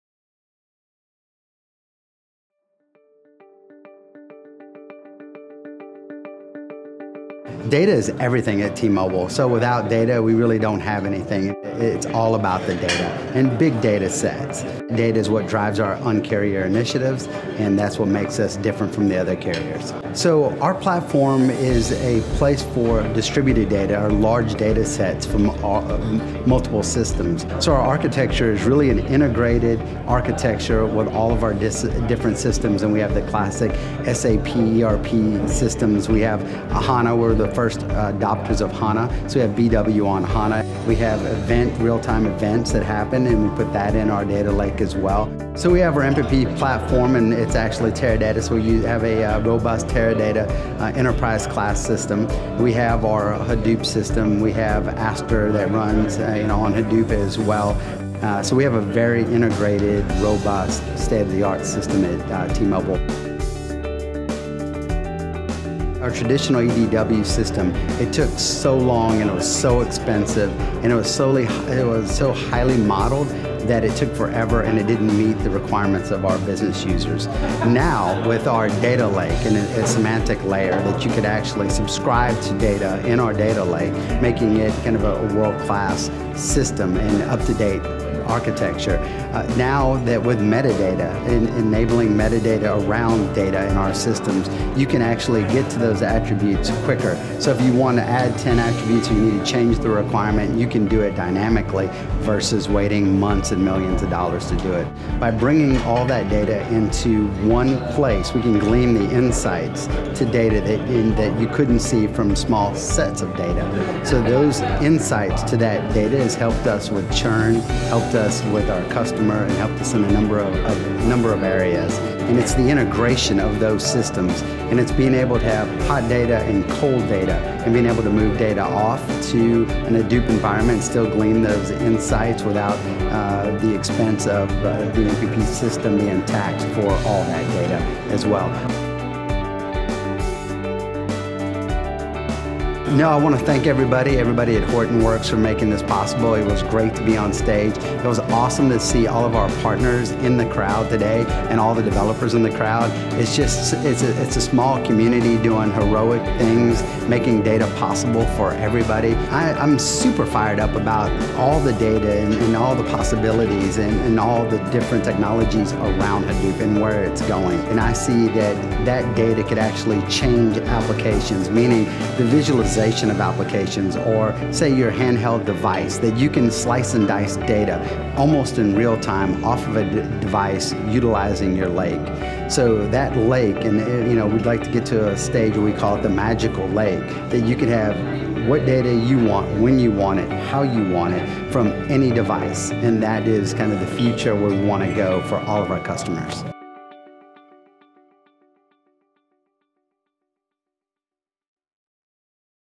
T-Mobile Achieves World-Class System for Customer Data and Actionable Insights with Hortonworks
For “Un-carrier" T-Mobile, data is everything. T-Mobile's previous traditional system was slow, highly modeled and expensive. It did not meet the needs of business users. Sean Thorne, Director of Business Intelligence, details how data is everything at T-Mobile. With Hortonworks, T-Mobile is able to quickly glean new customer insights that previously could not be seen before. This means they can have access to the right data, at the right time, and deliver it to the right people.